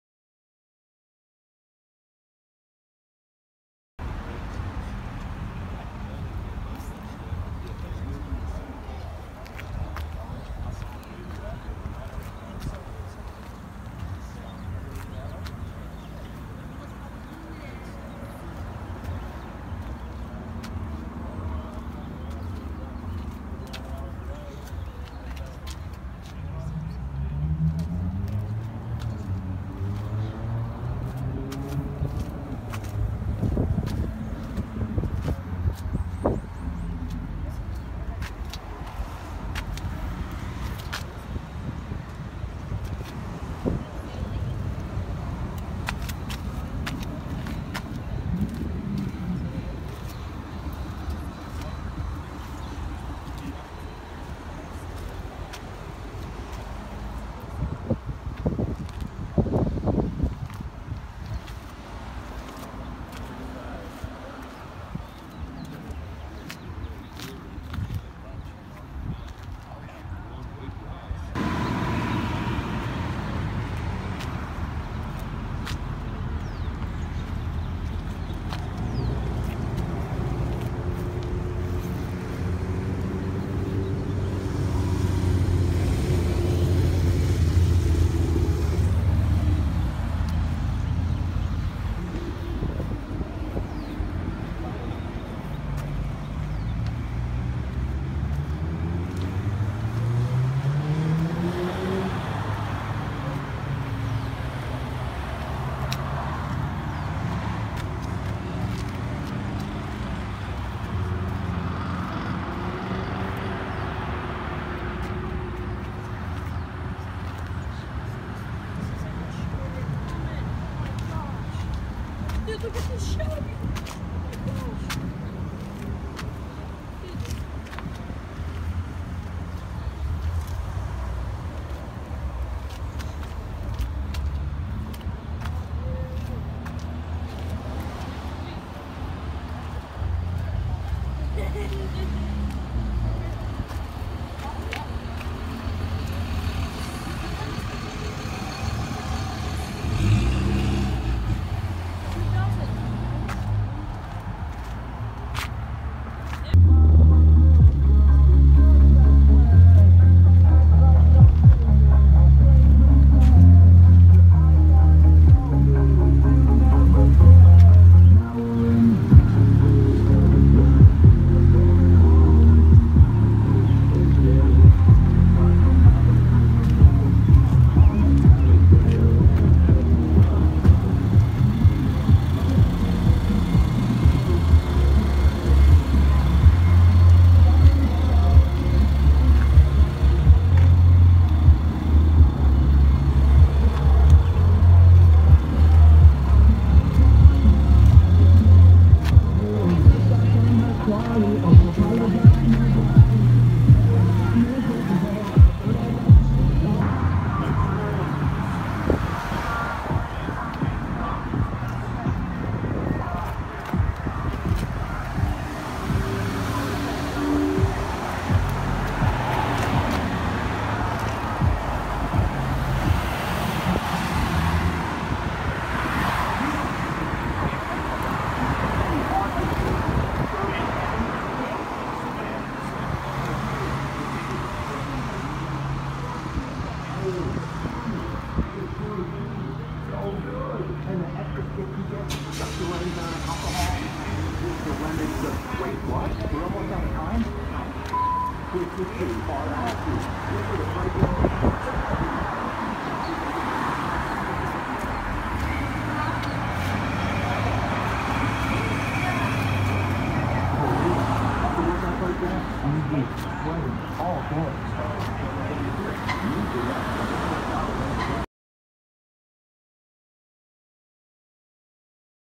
Historic downtown cruise in at Caves springs 2021
They canceled it last year cause of this year but this year Finally got a good documentary of the cars I saw that night